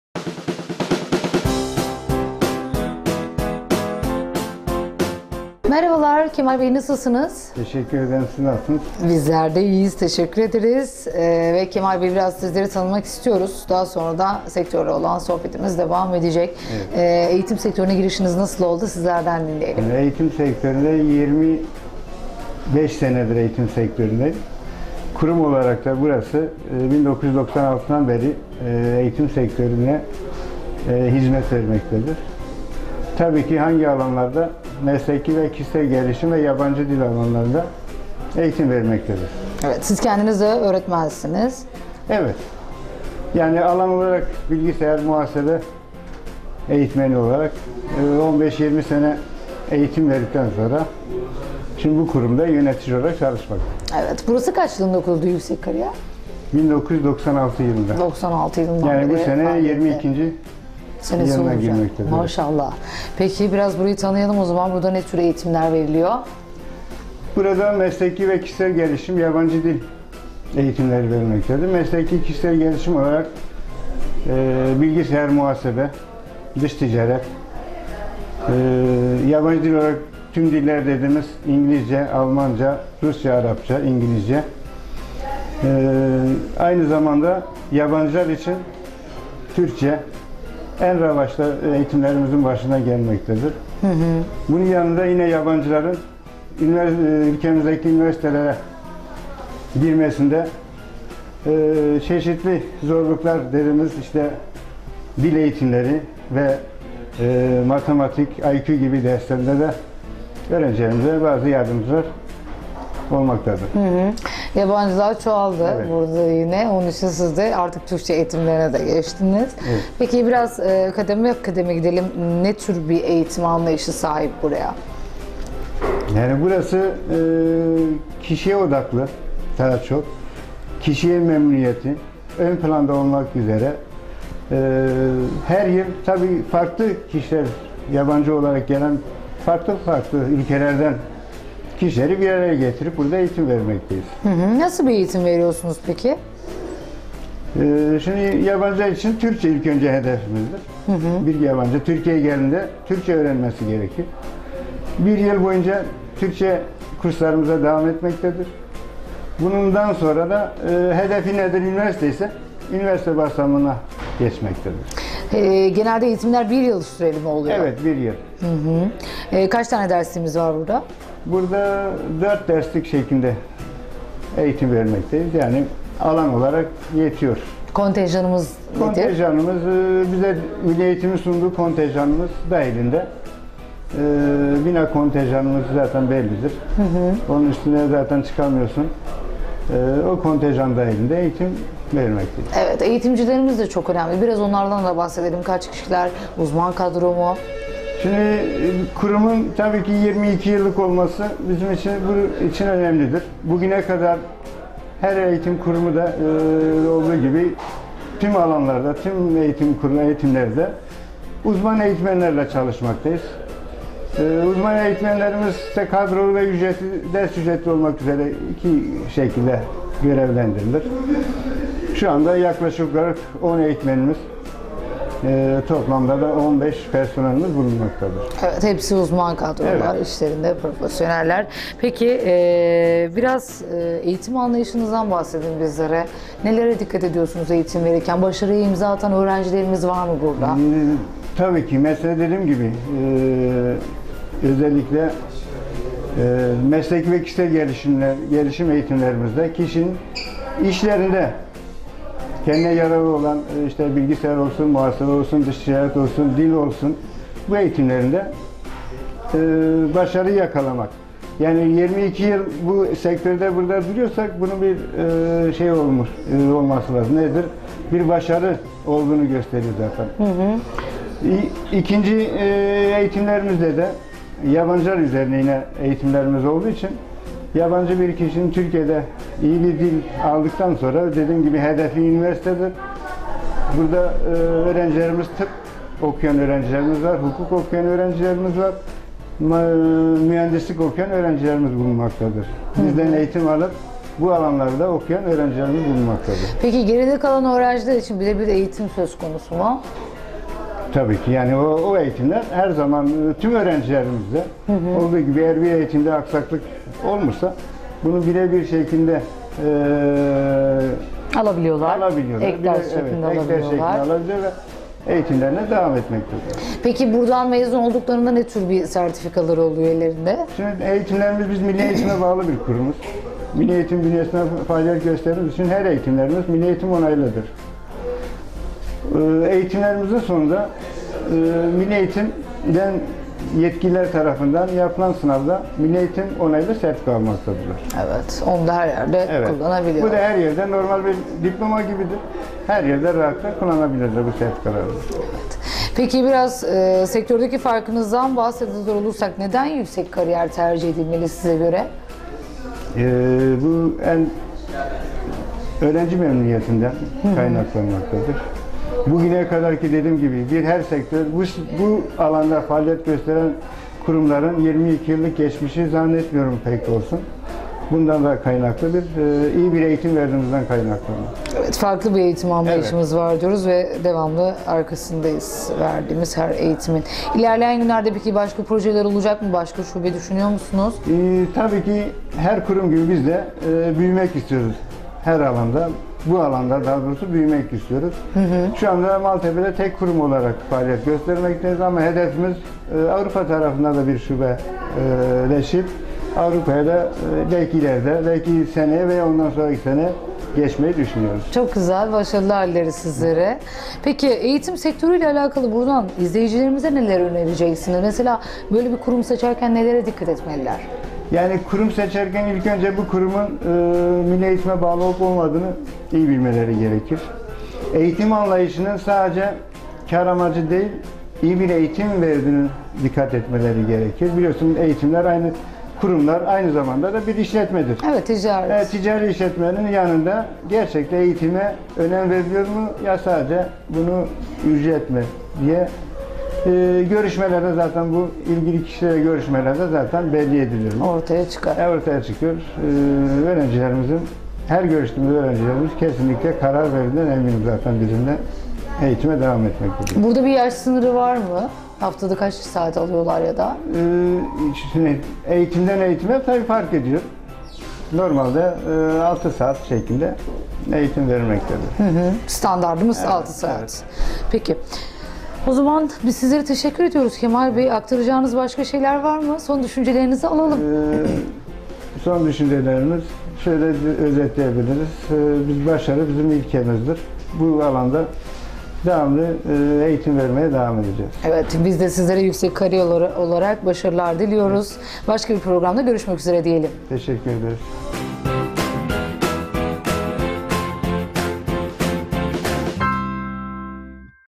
YÜKSEK KARİYER YABANCI DİL MESLEKİ VE KİŞİSEL GELİŞİM KURSU - MALTEPE/İSTANBUL
YÜKSEK KARİYER YABANCI DİL MESLEKİ VE KİŞİSEL GELİŞİM KURSU işletmesine kobi özel olarak konuk olduk